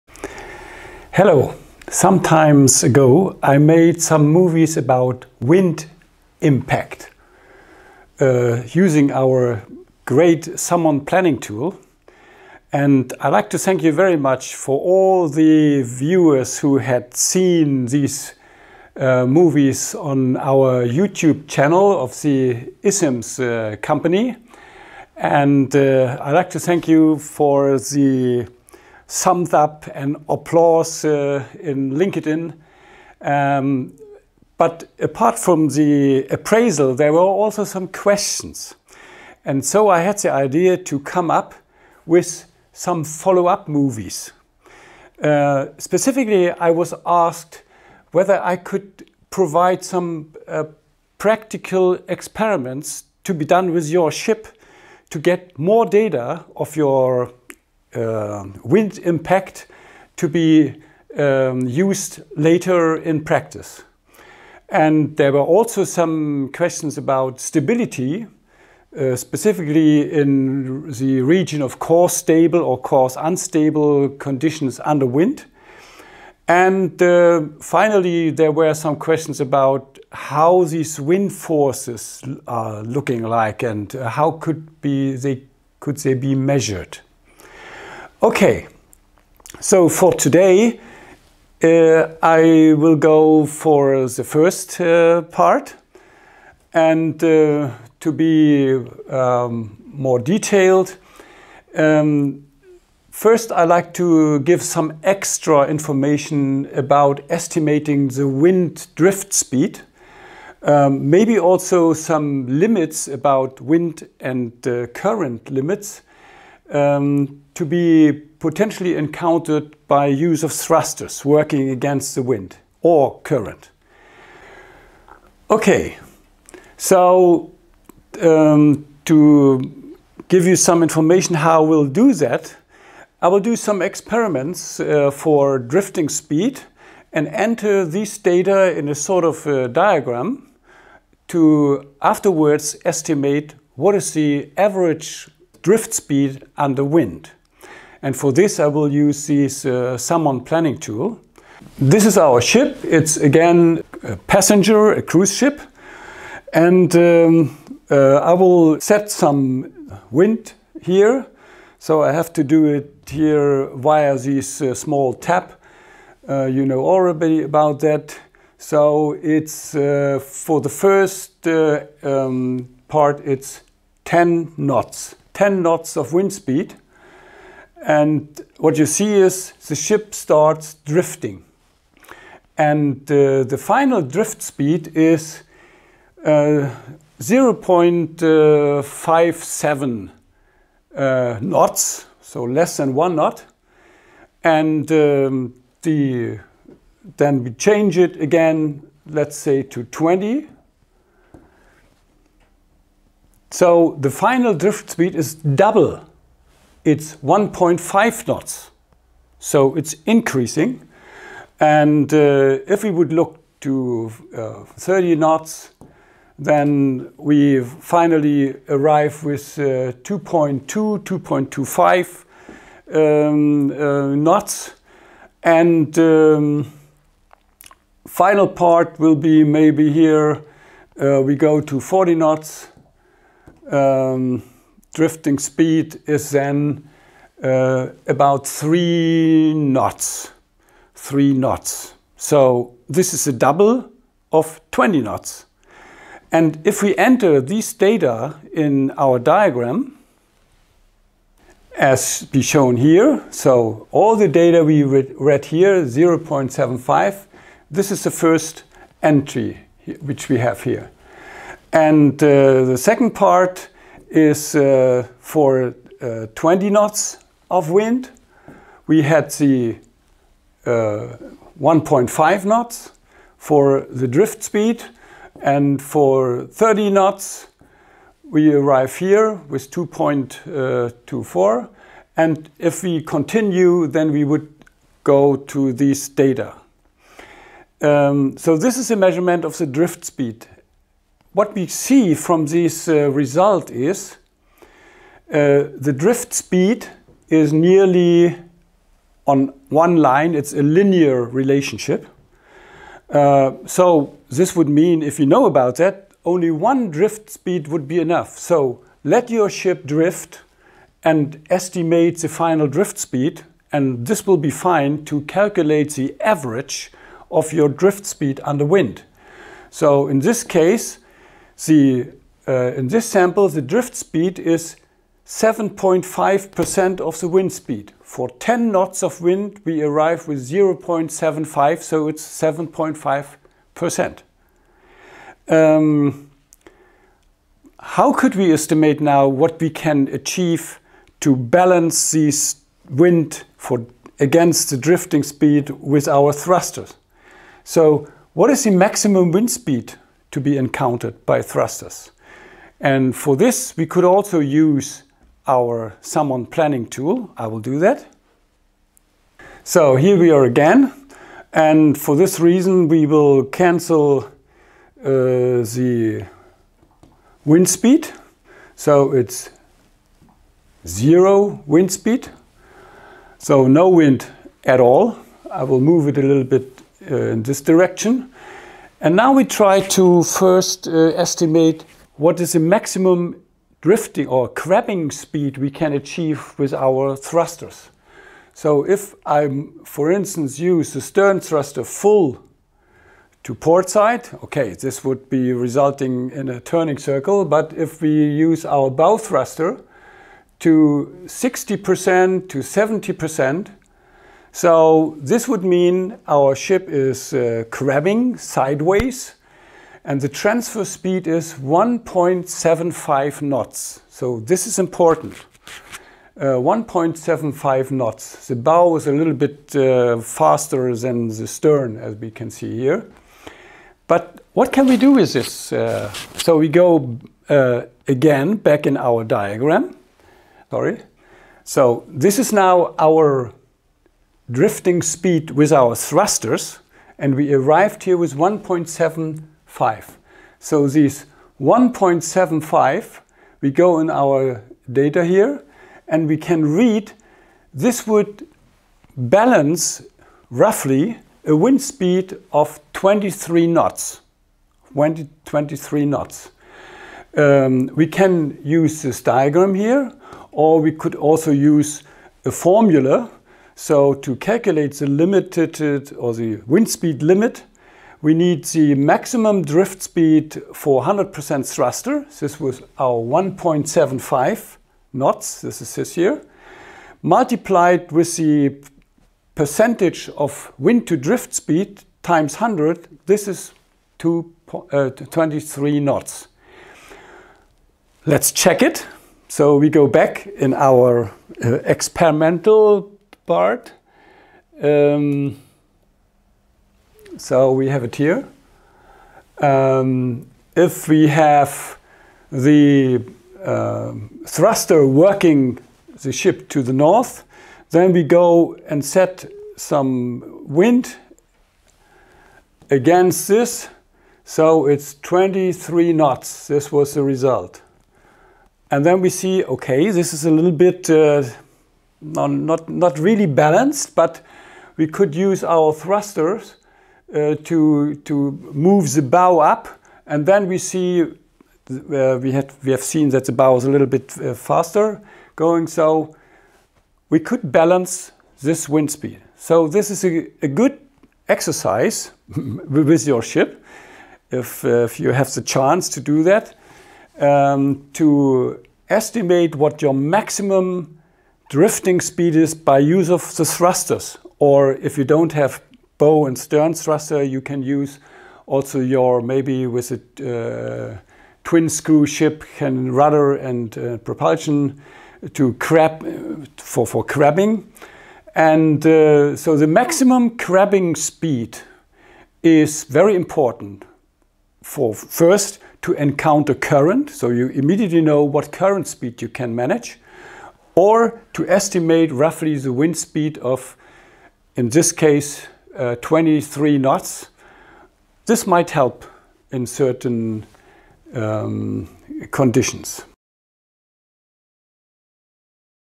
Wind pure drift encounter - practical experiments for getting useful data
How to get information for wind & current  limits to be potentially  encountered by thrusters – or current? - this will be described in this movie:
- Measure Drift speed, due to beam wind with no propulsion;
- Measure drift speed using full thrusters
- Estimate wind & current  limits to be potentially  encountered by thrusters – or current...
- Finally there is a simple formula as Rule of Thumb: the transverse drift speed is about 7-8% of wind speed!
And all this is demonstrated by means of the unique SAMMON Planning software based on Fast Time Simulation of ship manoeuvres...
Enjoy, share and give some thumbs up, if you are satisfied!
Knud Benedict